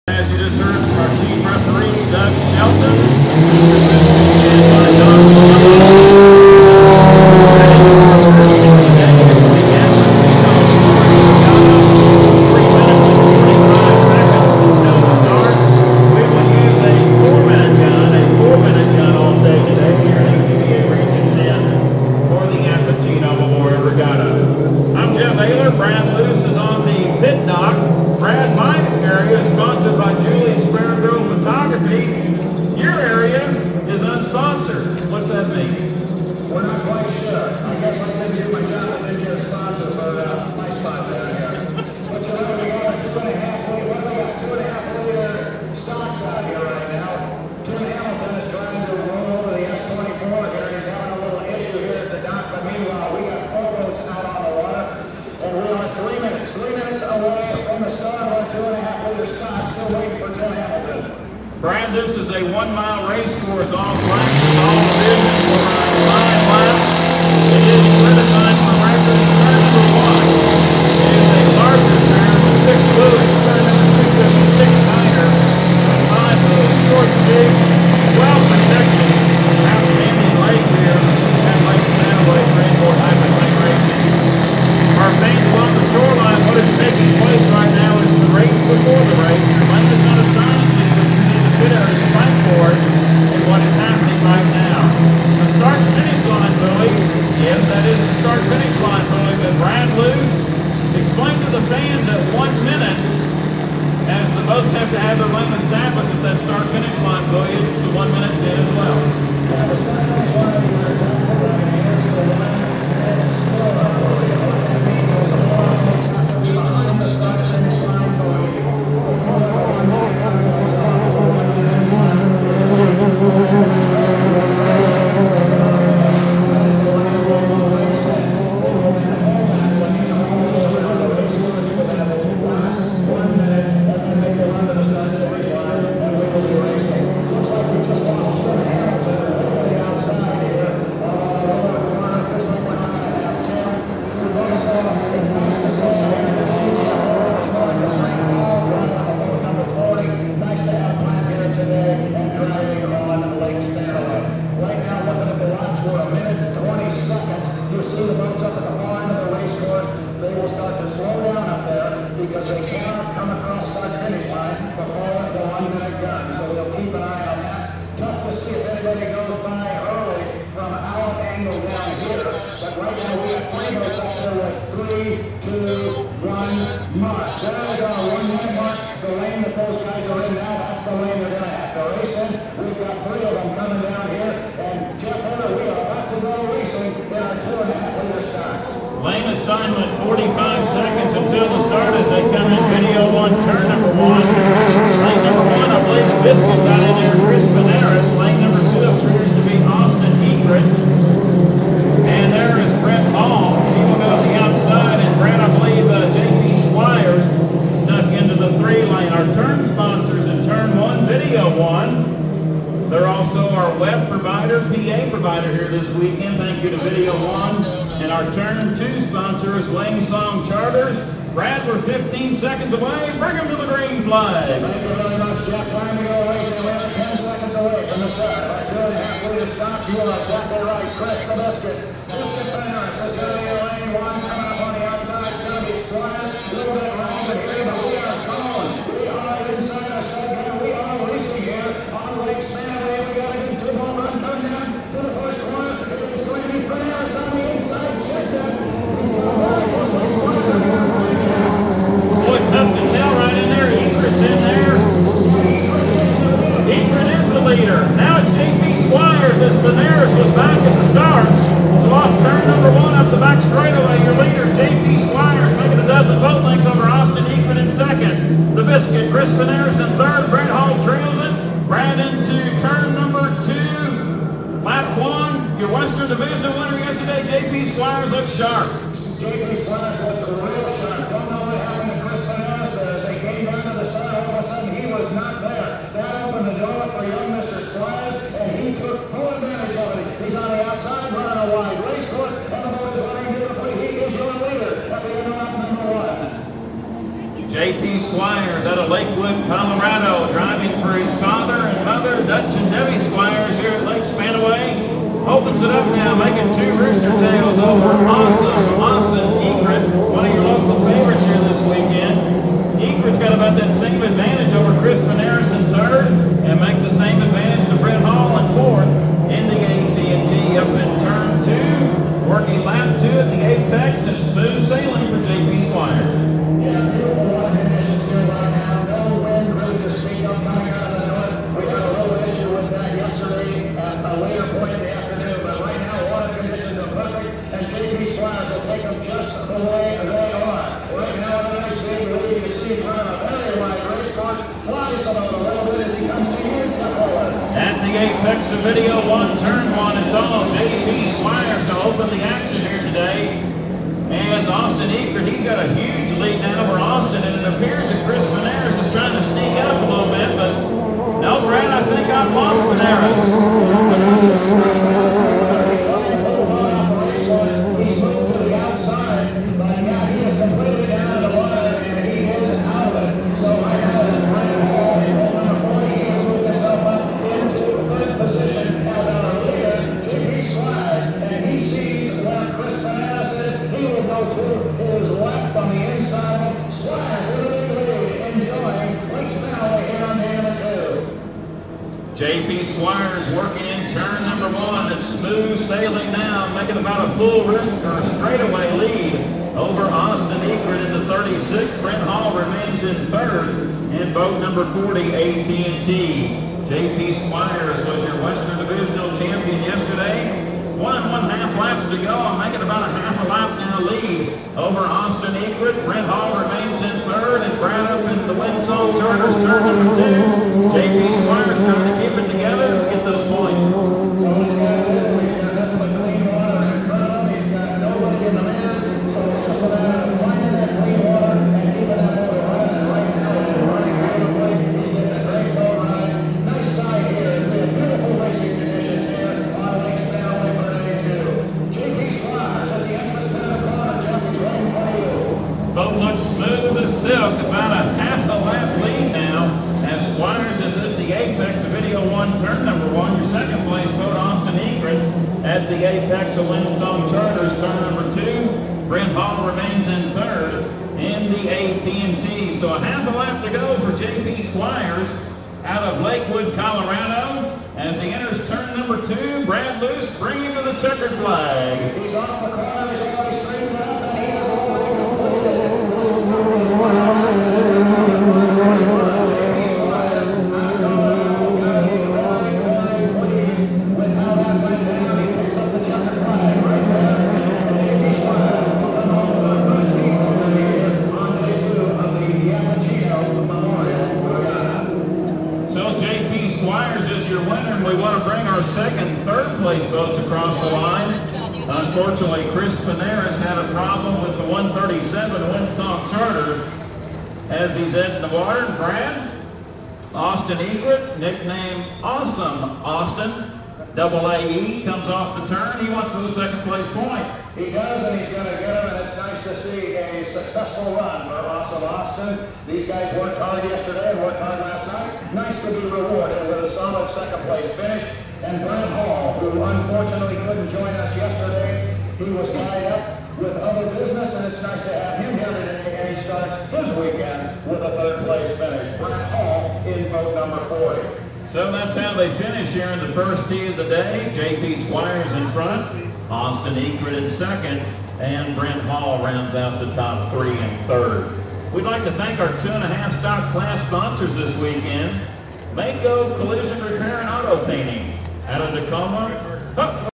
2011 Spanaway - Sunday - 2.5 Stock Heat 1
Scott and Linda Carson present,  The 2011 Western Divisionals, Yapachino Memorial Regatta at Spanaway Lake